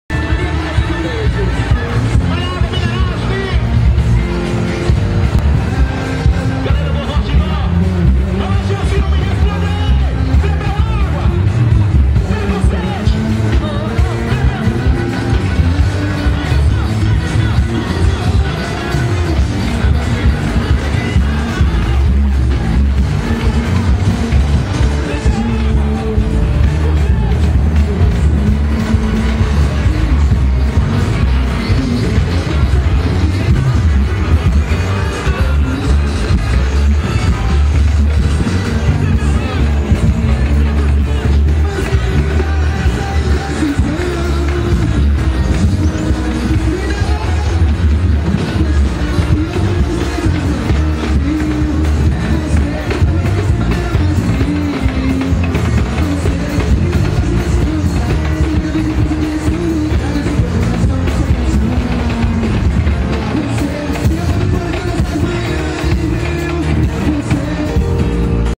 Colégio da Luz sai com bloco de carnaval Vaga-lume nesta quinta, 12 (2 de 3)
Nesta quinta-feira, 12 de fevereiro de 2015, o Colégio da Luz saiu com seu bloco Vaga-lume no centro de Guarabira. O bloco de carnaval composto por alunos e professores do colégio desfilou pelo centro da cidade com muita diversão e alegria.